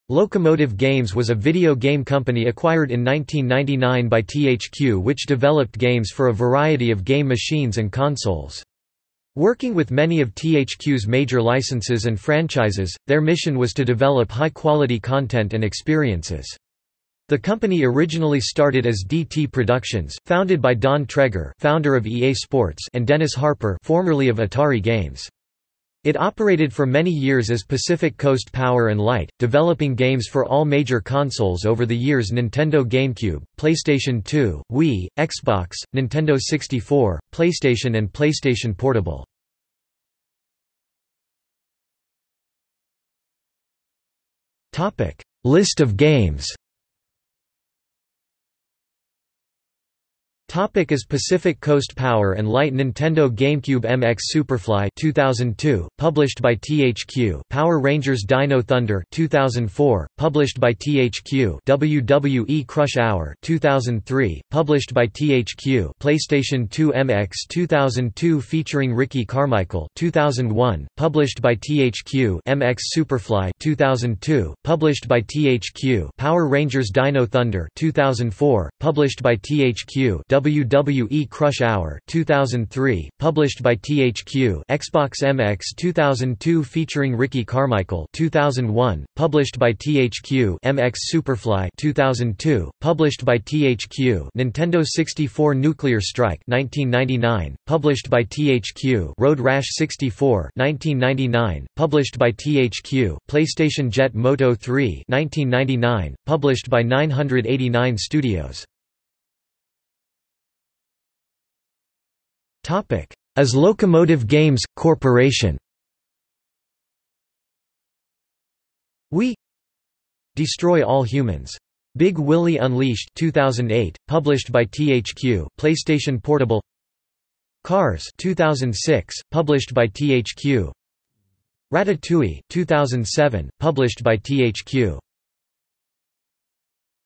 Locomotive Games | Wikipedia audio article
00:00:41 List of games
00:00:50 As Pacific Coast Power & Light
00:01:49 As Locomotive Games/Corporation

- Socrates

SUMMARY
Locomotive Games was a video game company acquired in 1999 by THQ which developed games for a variety of game machines and consoles. Working with many of THQ's major licenses and franchises, their mission was to develop high quality content and experiences. The company originally started as DT Productions, founded by Don Traeger (founder of EA Sports) and Dennis Harper (formerly of Atari Games). It operated for many years as Pacific Coast Power & Light, developing games for all major consoles over the years Nintendo GameCube, PlayStation 2, Wii, Xbox, Nintendo 64, PlayStation and PlayStation Portable.